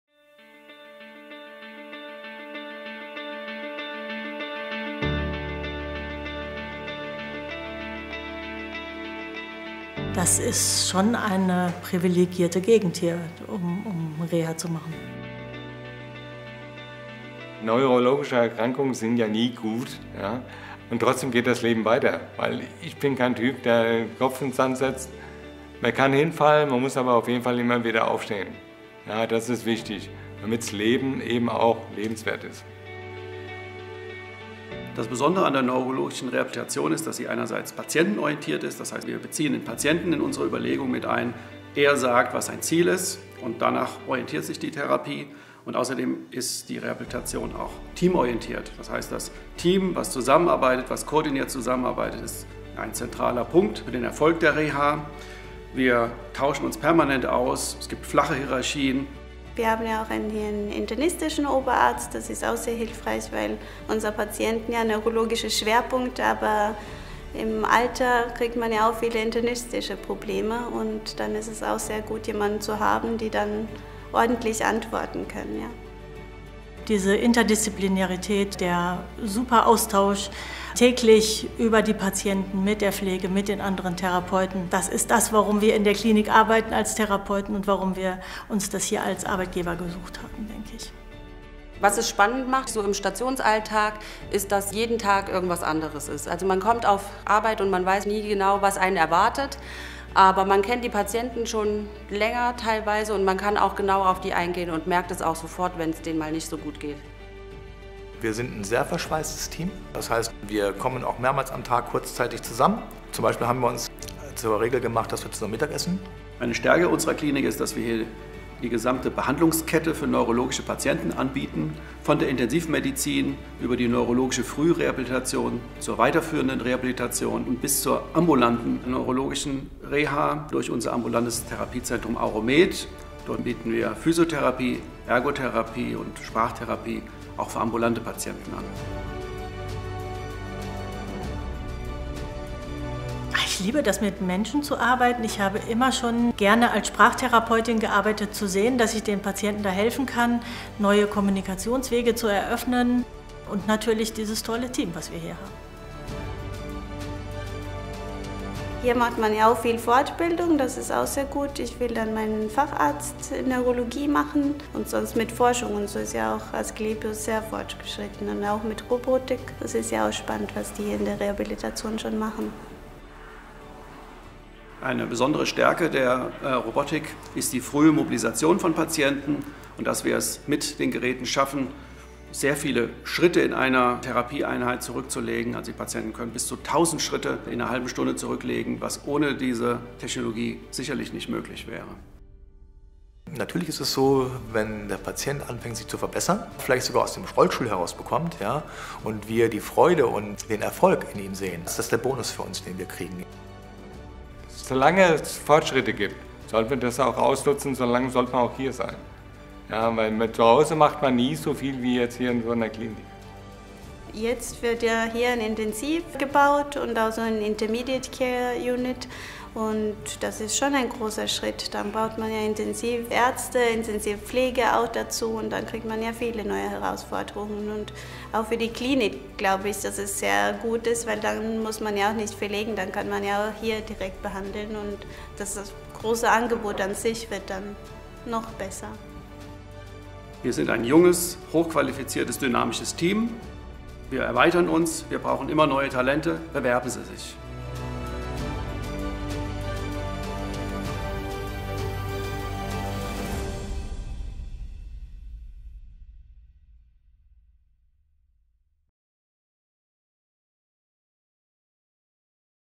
Neurologische Klinik Falkenstein | Asklepios
In der Neurologischen Klinik Falkenstein können Menschen nach einem Schlaganfall oder einer Schädigung des Gehirns durch einen Unfall oder einer anderen Erkrankung auch nach der Akuttherapie weiter behandelt werden. Denn gerade bei neurologischen Ausfällen oder Folgeschäden ist eine anschließende Reha von enormer Bedeutung. Zumal sich die interdisziplinäre Therapie in der Neurologischen Klinik Falkenstein an den Zielen der Patienten orientiert. Im Zentrum steht immer die schnelle Mobilisation. Dabei bietet die Klinik die gesamte Behandlungskette für neurologische Patienten von der Intensivmedizin, über die neurologische Frühreha zur weiterführenden Rehabilitation bis hin zur ambulanten neurologischen Rehabilitation. Das heißt, alles bleibt in einer Hand!